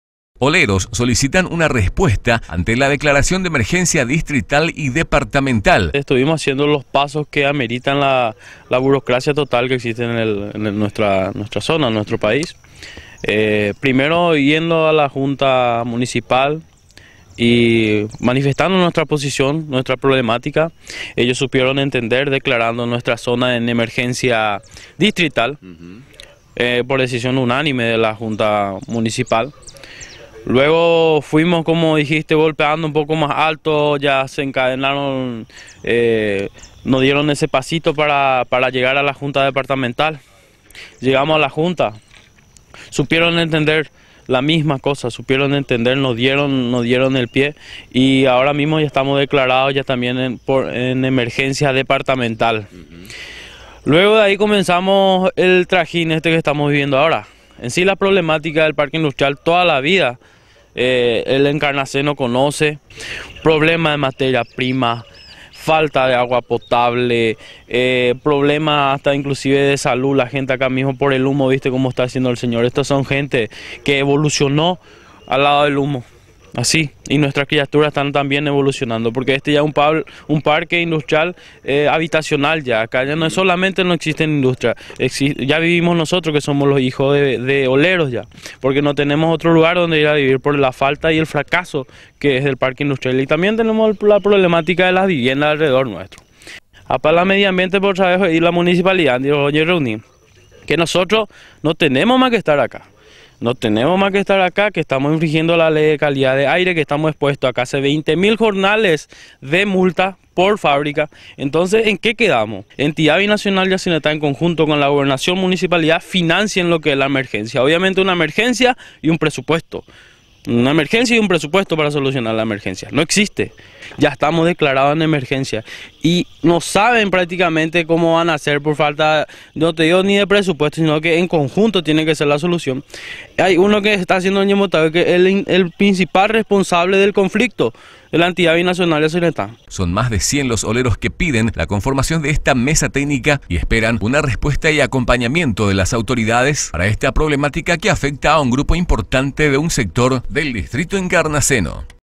OLEROS EXPECTANTES A RESPUESTA DEL GOBIERNO
Son más de 100 oleros que piden la conformación de la mesa técnica, han recibido la visita de representantes de la Municipalidad de Encarnación, la Junta Departamental quienes están al tanto de la problemática, abogan por respuesta de la Entidad Binacional